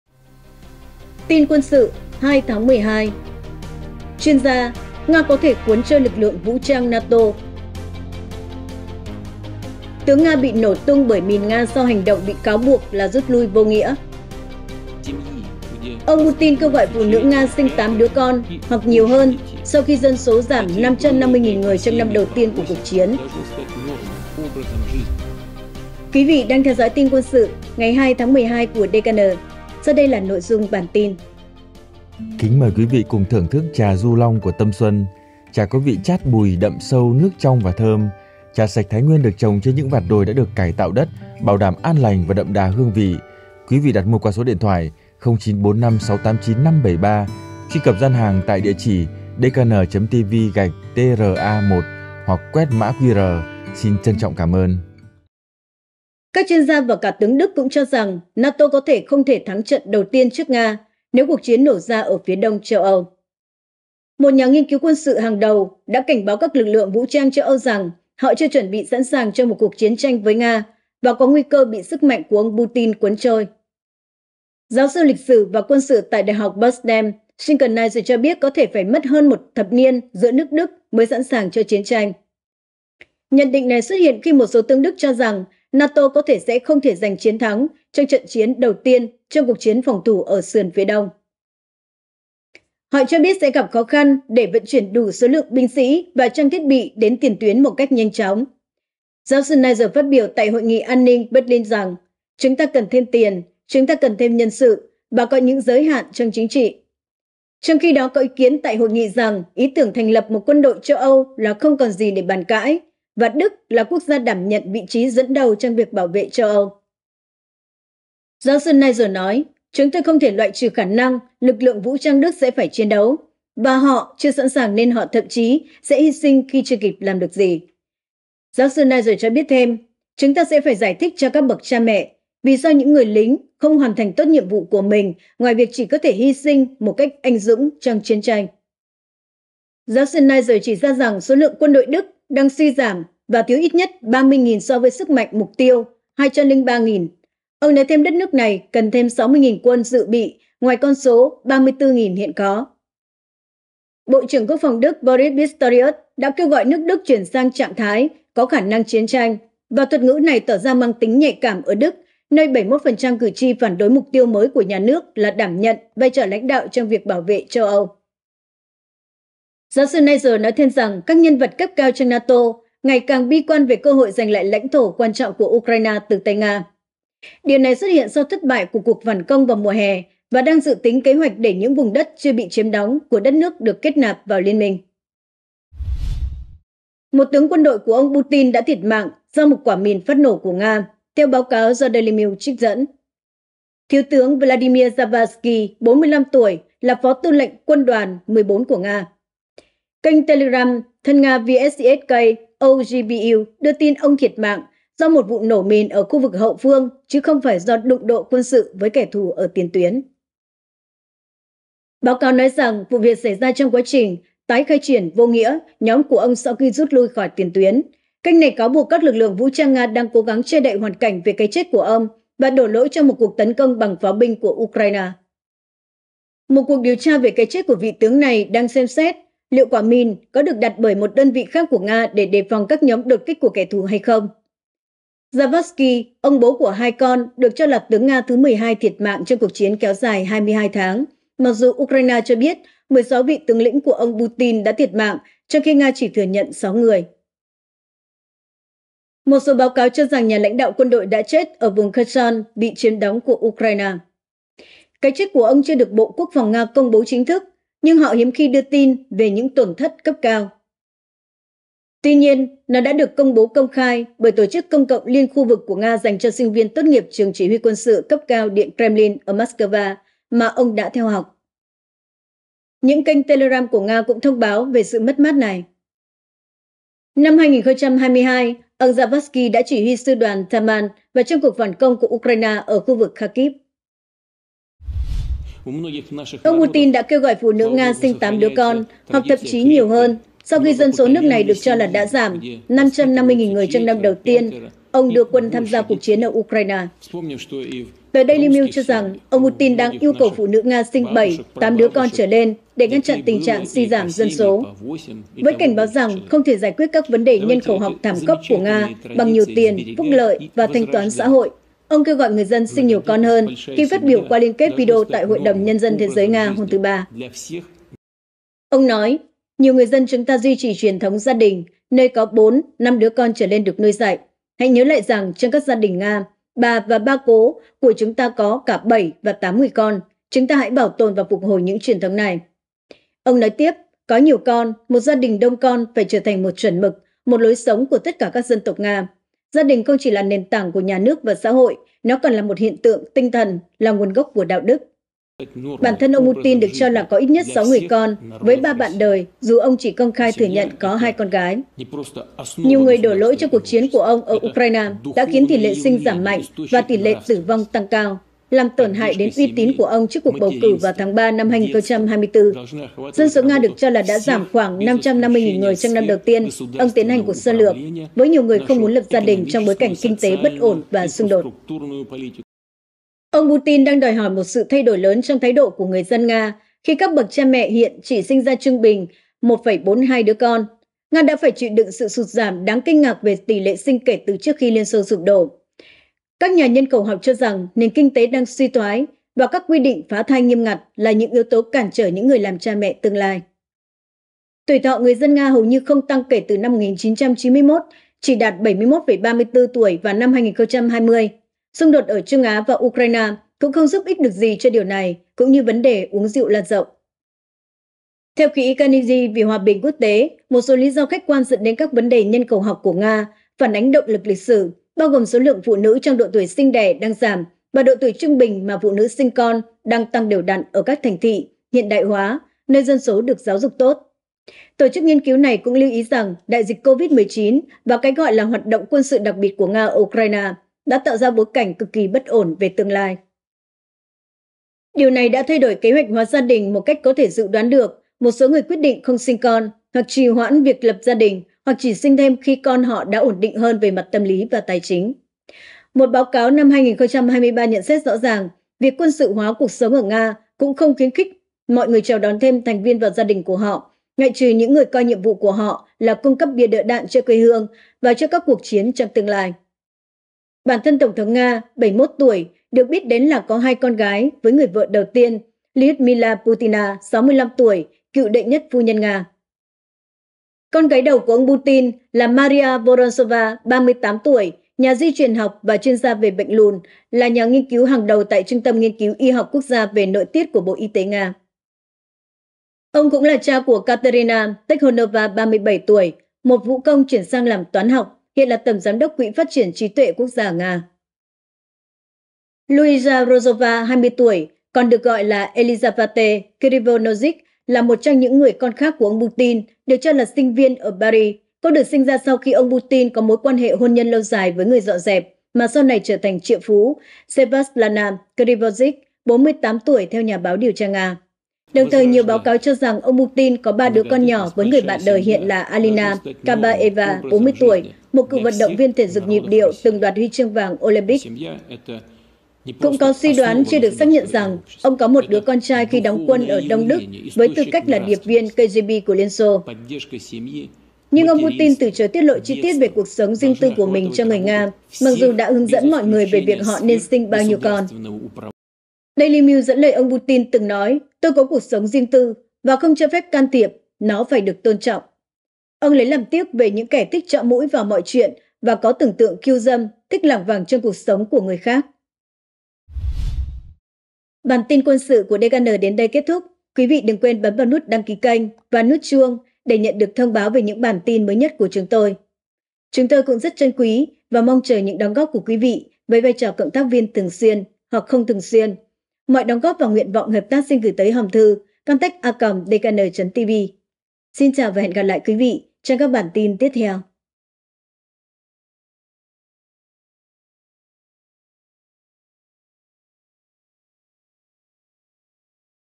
Ông Putin kêu gọi phụ nữ Nga sinh tám con sau khi dân số giảm trong năm đầu tiên của cuộc chiến
Tin Quân sự  2/12:
- Chuyên gia: Nga có thể 'cuốn trôi' lực lượng vũ trang NATO
- Tướng Nga bị nổ tung bởi mìn Nga sau hành động bị cáo buộc là rút lui 'vô nghĩa'
- Ông Putin kêu gọi phụ nữ Nga sinh tám đứa con hoặc nhiều hơn sau khi dân số giảm 550.000 người trong năm đầu tiên của cuộc chiến

Nếu quý vị có nhu cầu hợp tác công việc hay góp ý về nội dung, xin gửi email cho chúng tôi theo địa chỉ